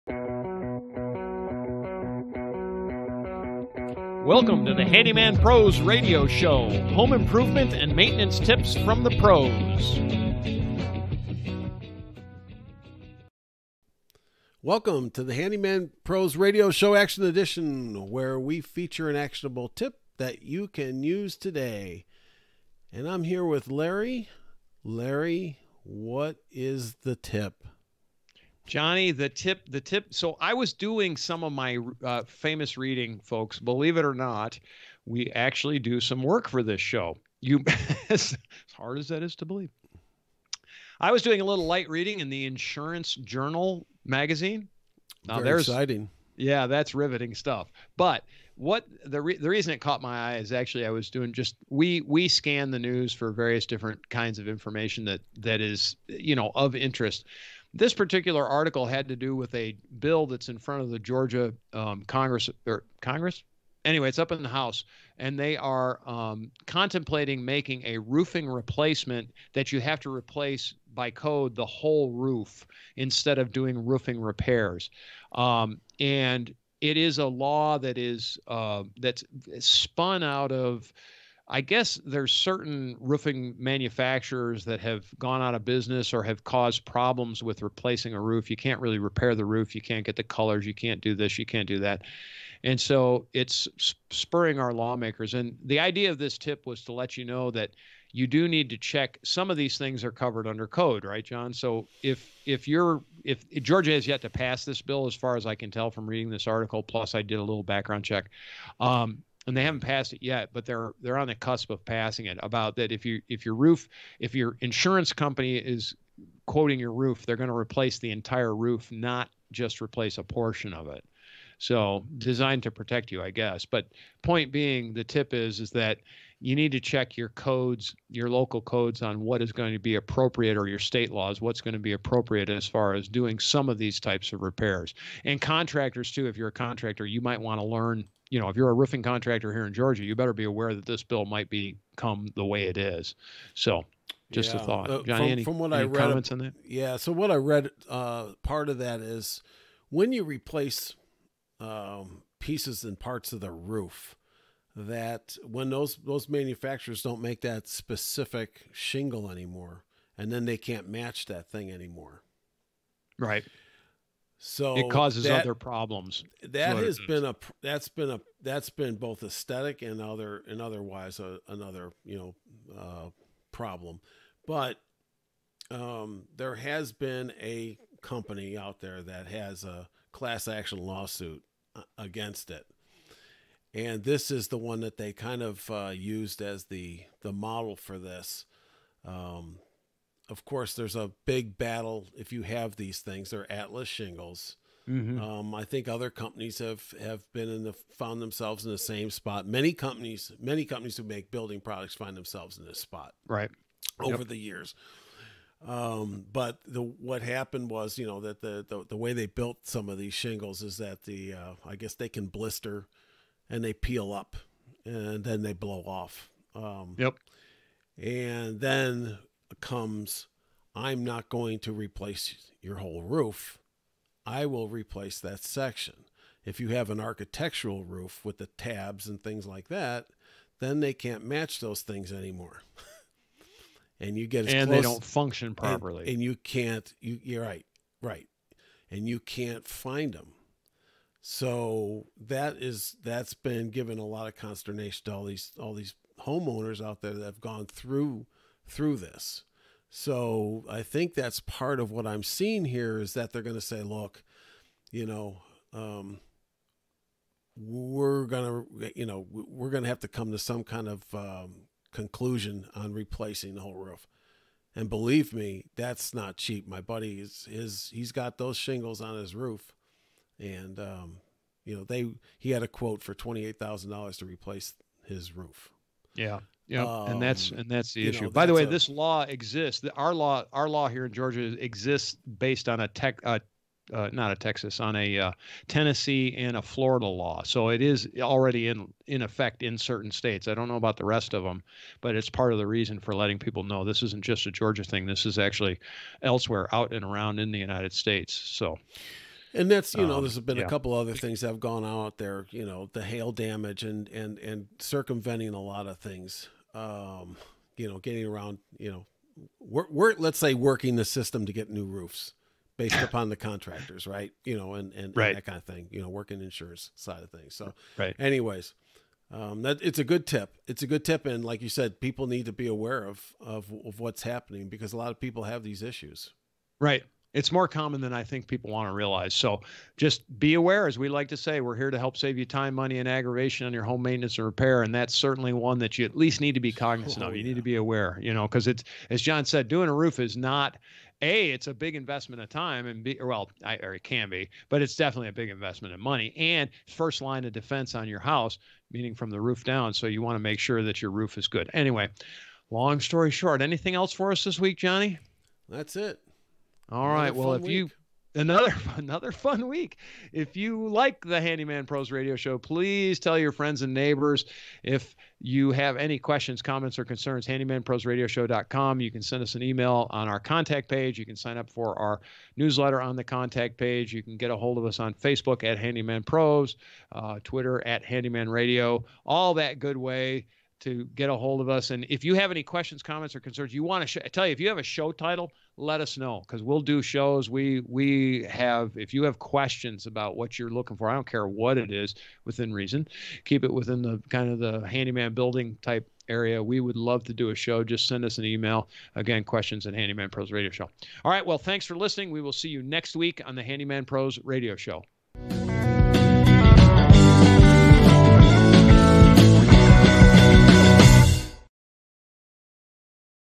Georgia Bill on Roofing Replacement
This episode discusses a proposed Georgia Bill that proposes that roofs need to be completely replaced when damaged.  Such laws already exist in other states.  It’s never a bad idea to check local regulations
Join our facebook group @handyman pros